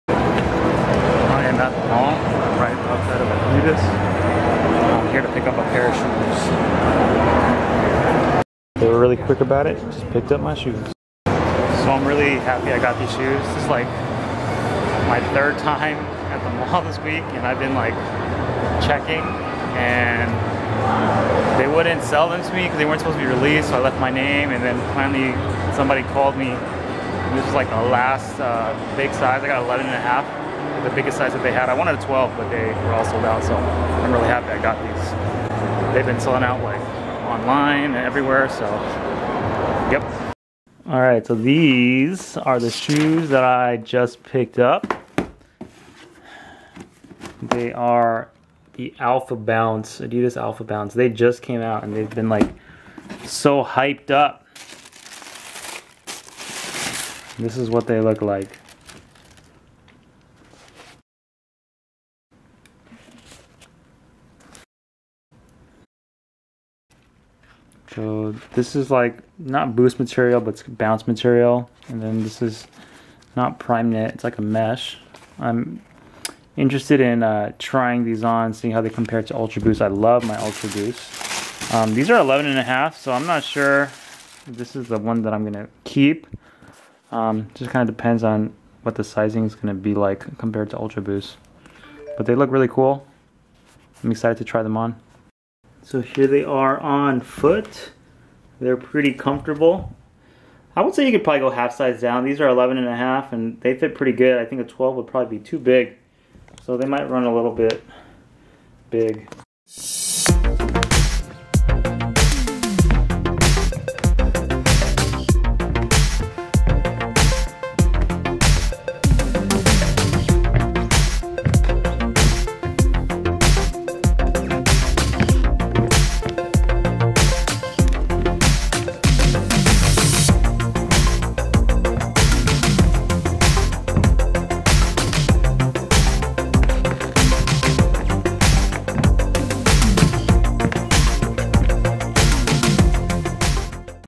Adidas Alpha Bounce Unboxing and On Foot Review Compared to Ultra Boost NMD Yeezy
We normally Daily Vlog but we like to post random videos like shoe unboxings and reviews too